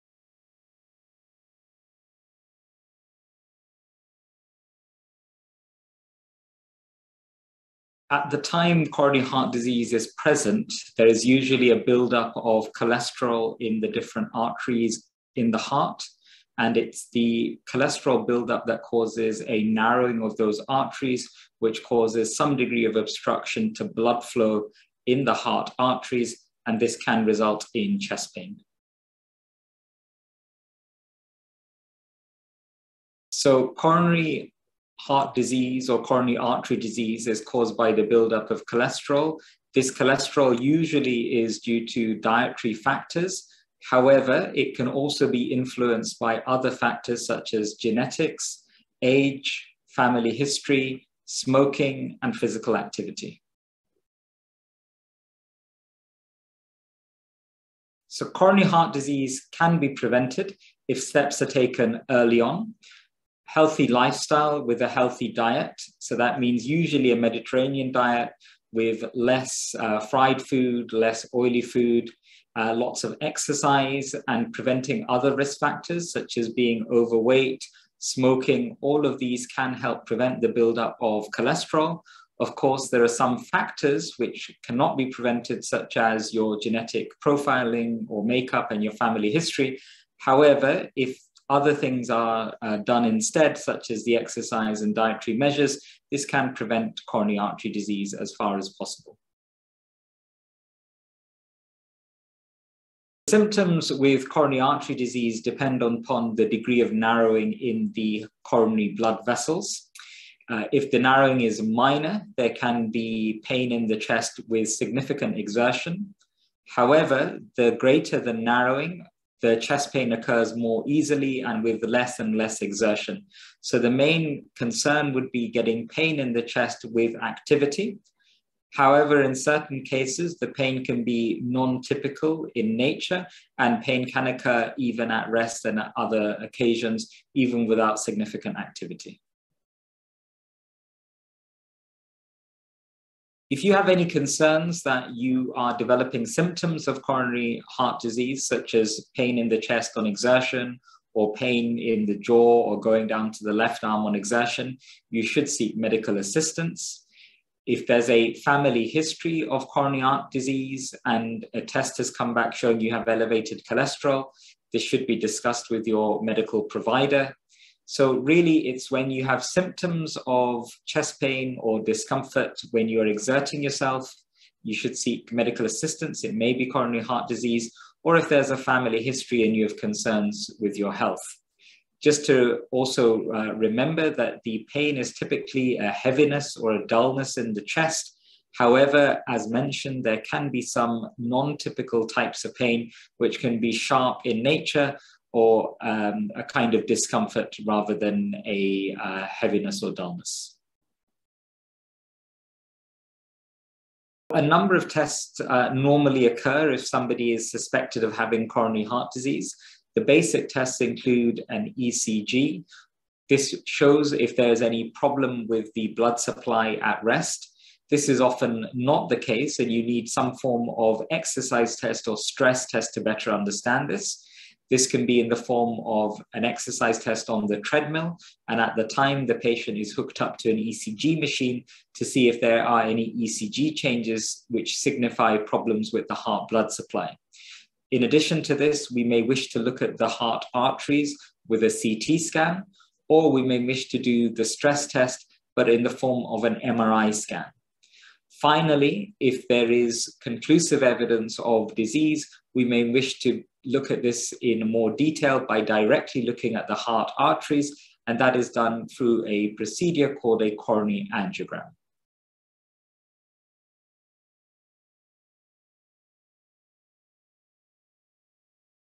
Coronary heart disease: symptoms, causes, and treatment - Online interview
Coronary heart disease occurs when the heart's arteries fail to deliver a sufficient amount of oxygen-rich blood to one's heart.

In our latest online interview, highly exprienced London-based consultant cardiologist, Dr Arjun Ghosh, details the symptoms and causes of coronary heart disease, before revealing what tests will be typically carried out on patients who present with these symptoms.

If you feel as though you are currently suffering from one or more of the various coronary heart disease-related symptoms mentioned in the video above, consult directly with Dr Ghosh today by visiting his Top Doctors profile